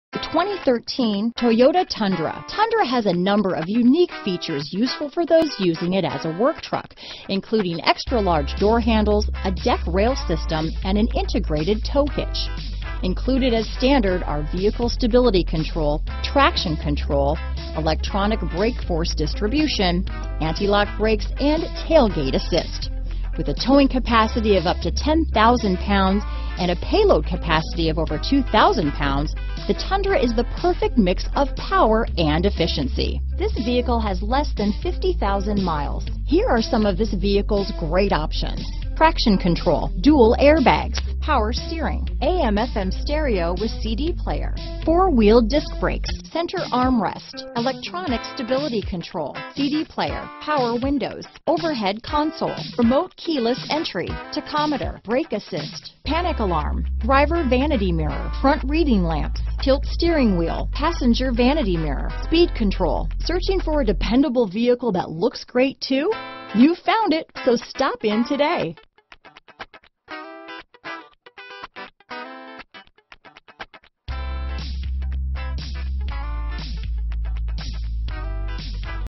2013 Toyota Tundra Lakeland, Lake Wales, Winter Haven, Kissimmee, Sebring, FL 39146274
Silver Sky Metallic U 2013 Toyota Tundra available in Lakeland, Florida at Weikert Ford. Servicing the Lake Wales, Winter Haven, Kissimmee, Sebring, FL area.

Weikert Ford
21399 US-27

Recent Arrival! Graphite Cloth. CARFAX One-Owner. 2013 Toyota Tundra Grade Silver Odometer is 28887 miles below market average! Priced below KBB Fair Purchase Price!